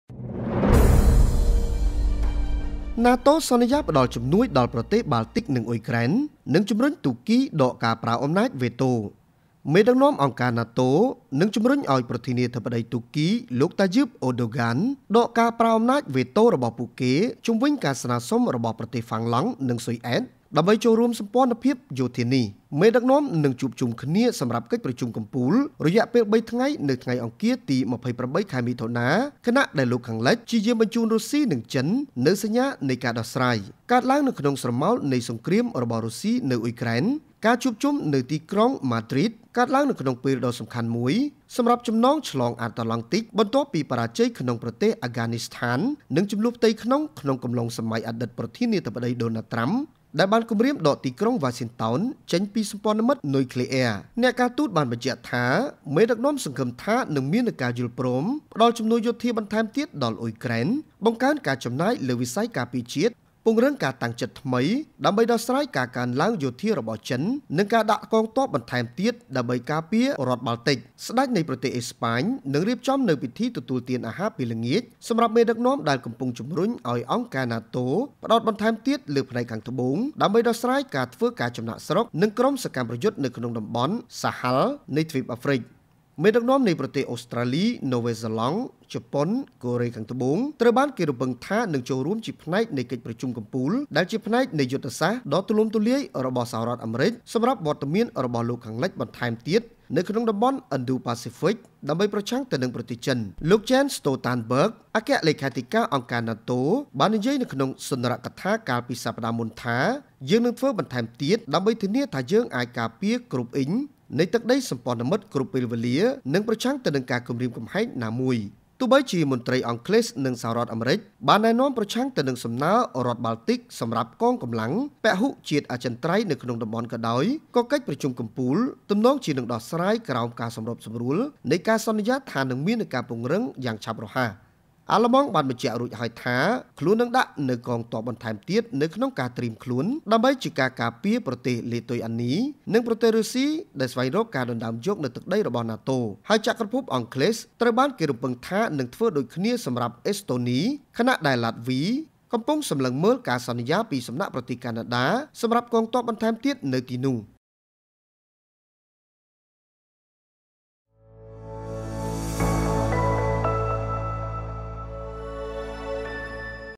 ណាតូ សន្យាផ្តល់ជំនួយដល់ប្រទេសបាល់ទិកនិងអ៊ុយក្រែន ហើយជំរុញតួកគីដកការប្រើអំណាចវេតូ | SBM NEWS
ណាតូ សន្យាផ្តល់ជំនួយដល់ប្រទេសបាល់ទិកនិងអ៊ុយក្រែន ហើយជំរុញតួកគីដកការប្រើអំណាចវេតូ